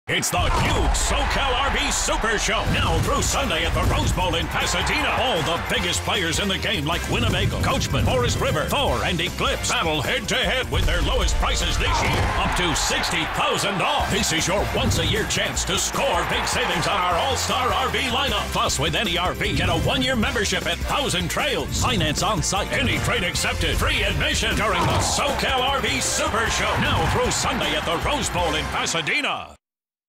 The Southern California RV Show at Rose Bowl Stadium in Pasadena, CA, Sponsored by McMahon's RV
Head on down to The Southern California RV Show at Rose Bowl Stadium in Pasadena, CA, August 16 - 25, 2013, and check out 100s of new and used RVs all at one location! Over a dozen manufacturers to choose from!